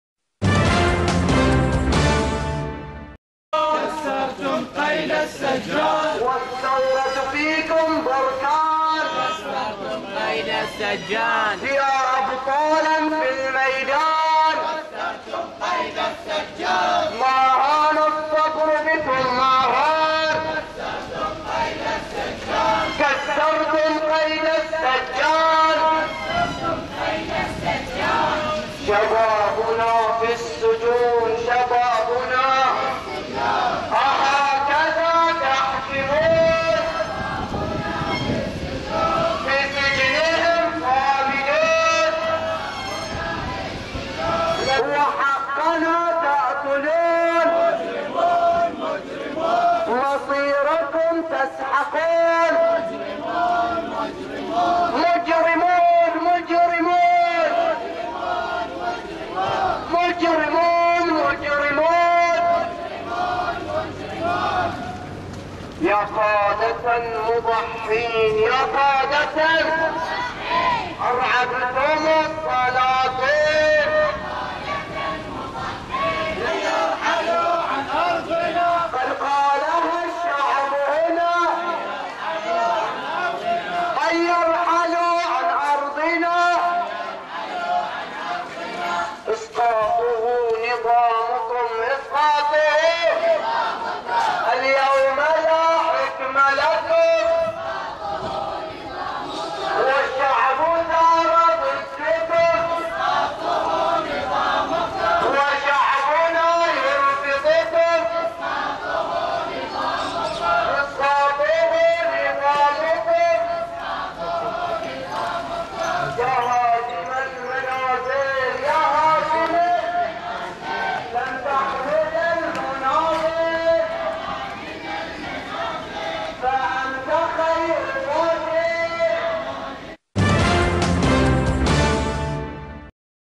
البحرين : مسيرة "نصرة الأوفياء" تضامنا مع المعتقلين - النويدرات 27/6/2013
خرجت عصر اليوم الخميس (27 يونيو/حزيران 2013) مسيرة جماهيرية بمنطقة النويدرات جنوب العاصمة المنامة تضامنا مع المعتقلين السياسيين، وتأكيداً على حقهم الثابت في الحرية، وشارك في المسيرة حشد من أهالي القرية حملوا أعلام البحرين ورددوا شعارات مناهضة للنظام الخليفي، وتأتي هذه المسيرة ضمن أسبوع "نصرة الأوفياء" الذي ينظمه شباب النويدرات في منطقتهم.